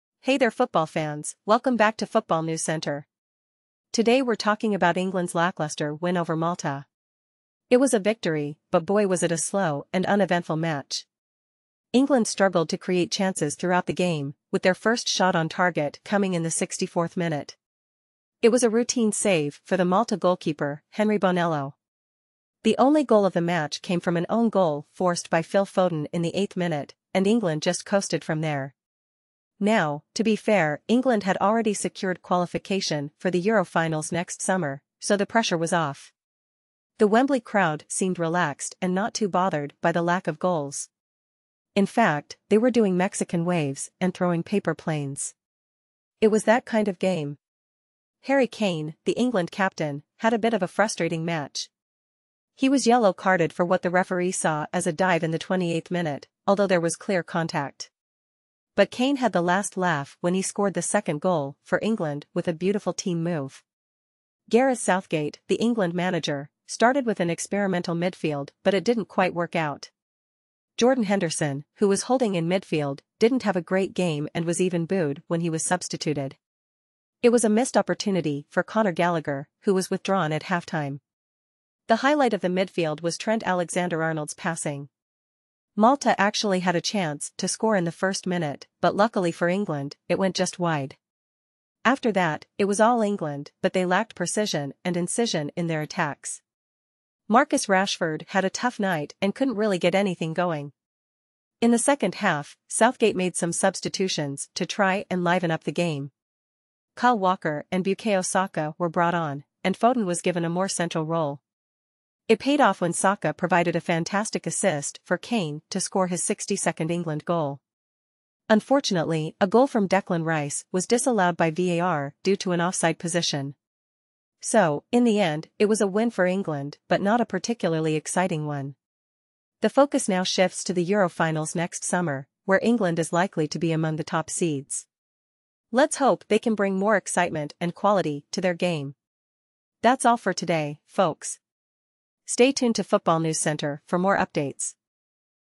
Harry Kane strikes in England’s lacklustre win over Malta
England secured a comfortable victory over Malta in their World Cup qualifier match, with a final score of 2-0. The game was largely lacklustre, with England only managing their first shot on target in the 64th minute. The opening goal came in the 8th minute when Phil Foden forced an own goal from Enrico Pepe. Harry Kane was incensed to be yellow carded for diving in the 28th minute, but he later scored the second goal of the match. The victory means that England has secured qualification for the Euro finals next summer and is likely to be among the top seeds for the group phase draw. The match also saw an experimental midfield lineup for England, with Jordan Henderson, Trent Alexander-Arnold, and Conor Gallagher starting. However, the midfield performance was underwhelming, and Henderson was substituted to the sound of boos from the crowd. Overall, the match lacked intensity and precision from England, but the victory was never in doubt.